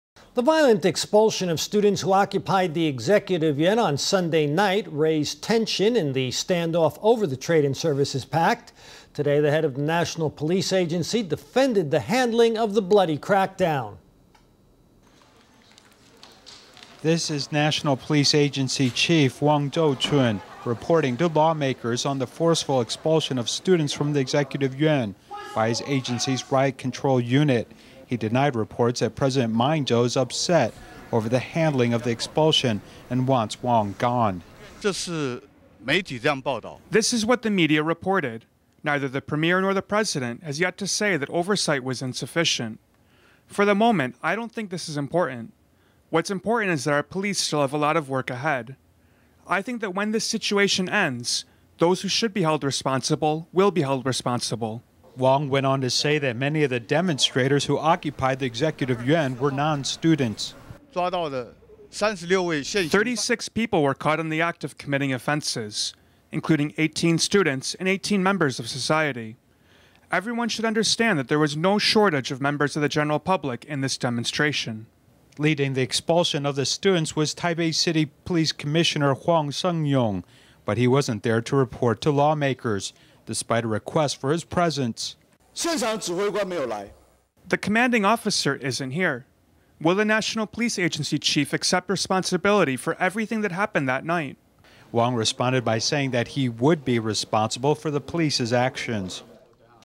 Head of National Policy Agency defends violent crackdown on students to angry lawmakers
The violent expulsion of students who occupied the Executive Yuan on Sunday night raised tension in the standoff over the trade-in-services pact. Today the head of the National Police Agency defended the handling of the bloody crackdown. This is National Police Agency Chief Wang Cho-chiun reporting to lawmakers on the forceful expulsion of students from the Executive Yuan by his agency's riot control unit. He denied reports that President Ma Ying-jeou is upset over the handling of the expulsion and wants Wang gone.Wang Cho-chiunNational Police Agency ChiefThis is what the media reported. Neither the premier nor the president has yet to say that oversight was insufficient. For the moment, I don't think this is important. What's important is that our police still have a lot of work ahead. I